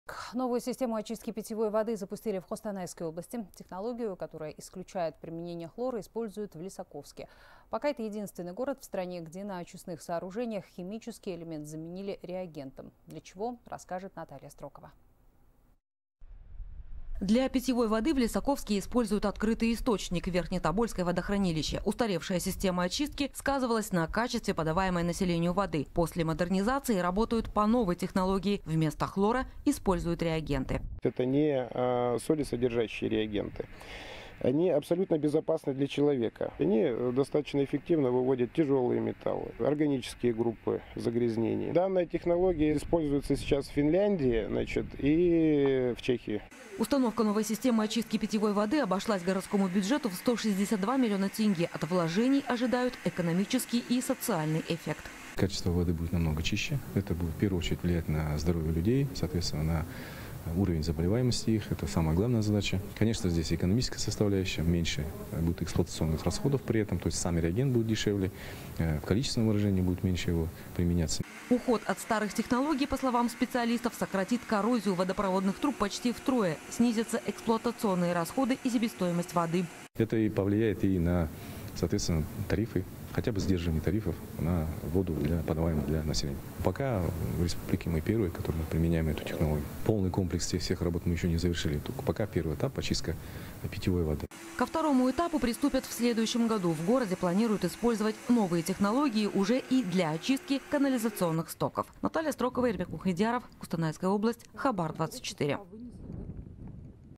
Новую систему очистки воды запустили в Костанайской области
Новую систему очистки питьевой воды запустили в Костанайской области. Технологию, которая исключает применение хлора, используют в Лисаковске.